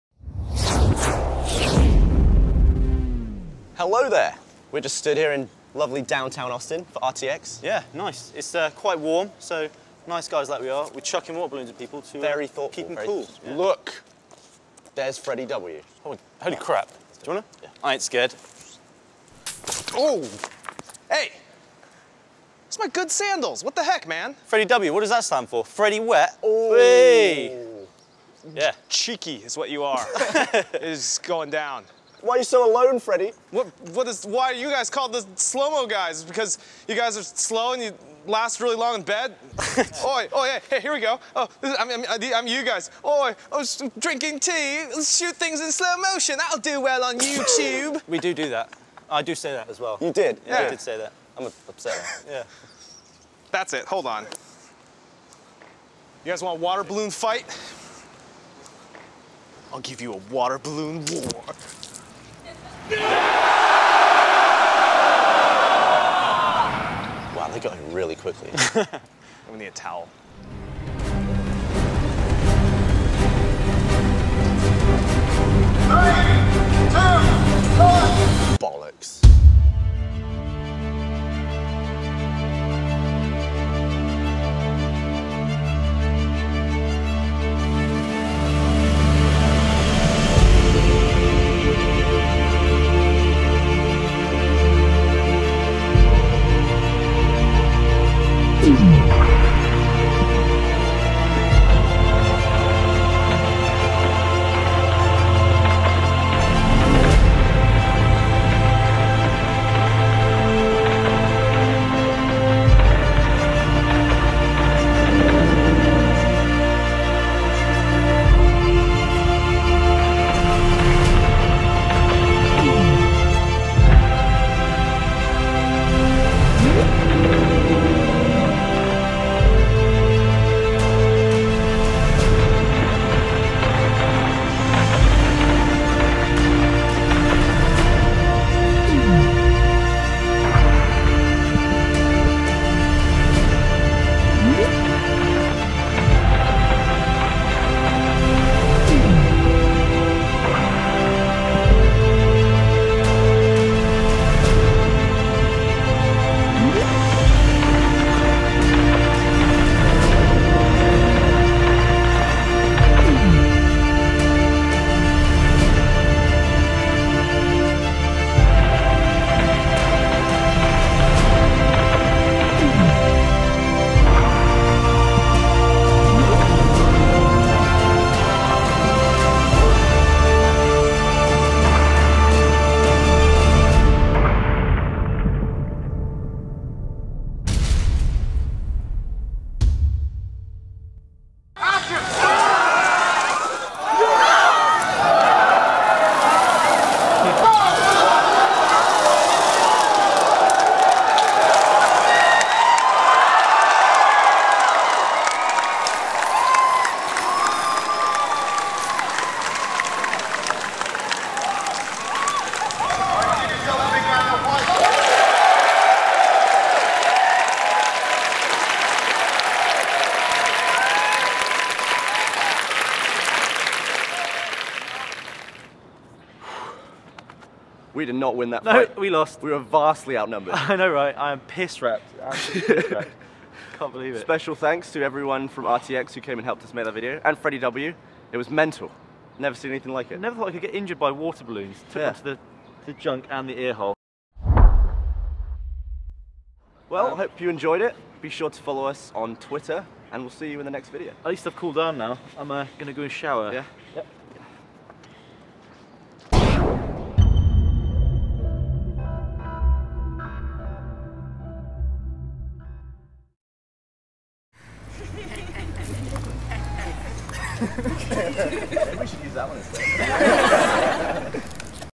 Slow Motion Water Balloon Fight with 1500 people Starring Freddie Wong
Gav and Dan start a water balloon fight with over a thousand people and film it at 2500fps. It does not end well for them.

Filmed with a Phantom Flex
Geek Week - Slow Motion Water Balloon Fight with 1500 people Starring Freddie Wong - The Slow Mo Guys